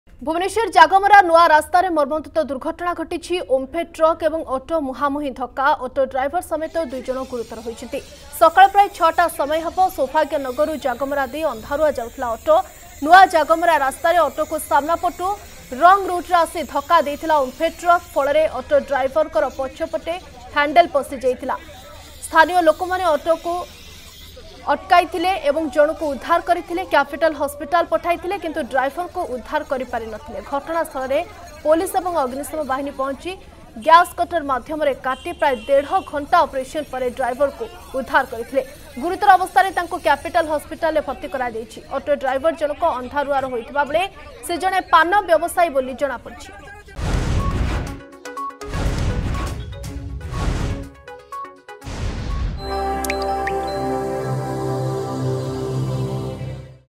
Jagamara, Bhubaneswar: 3 Injured In Omfed Truck-Auto Accident
ଭୁବନେଶ୍ବର ଜାଗମରା ନୂଆ ରାସ୍ତାରେ ମର୍ମନ୍ତୁଦ ଦୁର୍ଘଟଣା । ଓମ୍ପେଡ୍‌ ଟ୍ରକ ଓ ଅଟୋ ମୁହାଁମୁହିଁ ଧକ୍କା । ଅଟୋ ଡ୍ରାଇଭର ସମେତ ଦୁଇ ଗୁରୁତର ହୋଇଛନ୍ତି ।